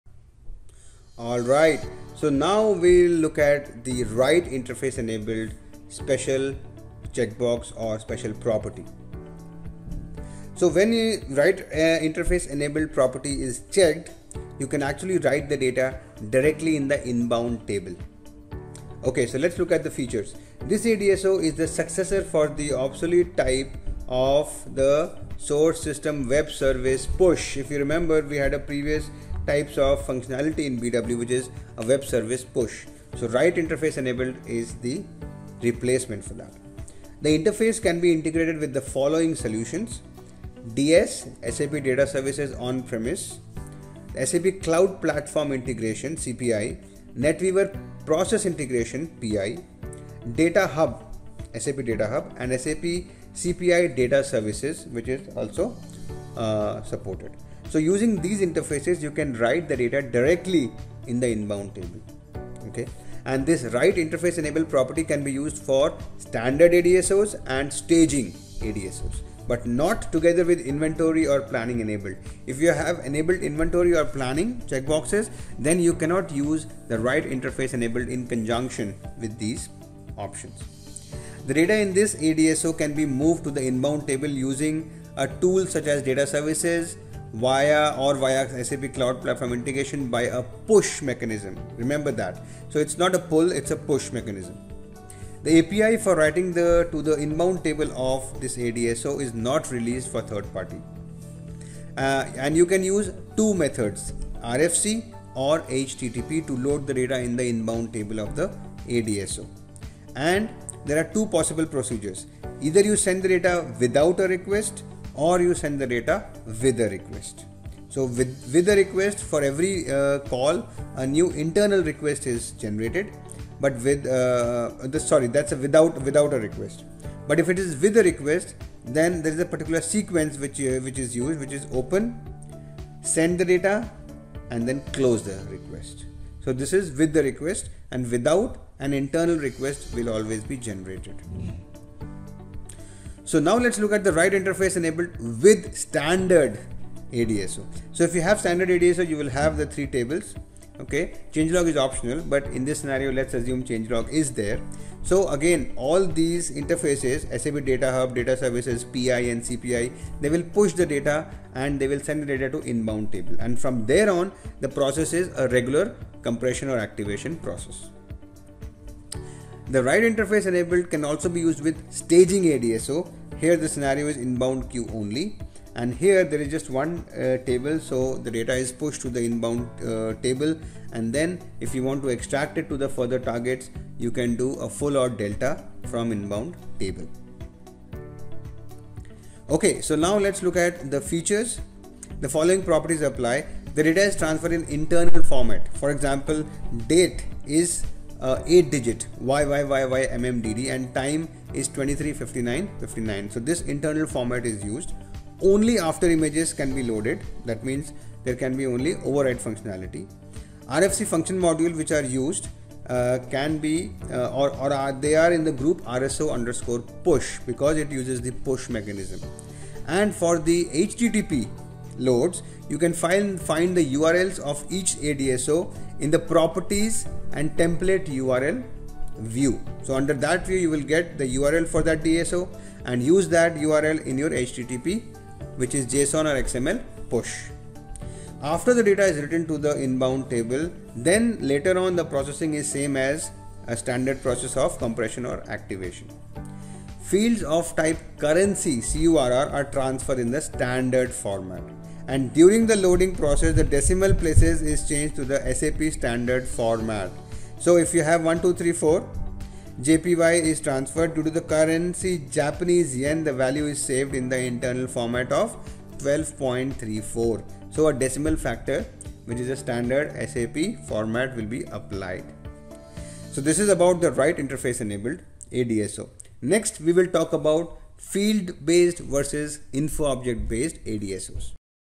Part 9 - Write enabled ADSO
Advanced DSO is the most important Infoprovider to persist transaction data and has replaced both classic DSO and InfoCube of the past, including the real-time and planning enabled Cubes.

This video explains the Write-enabled ADSO.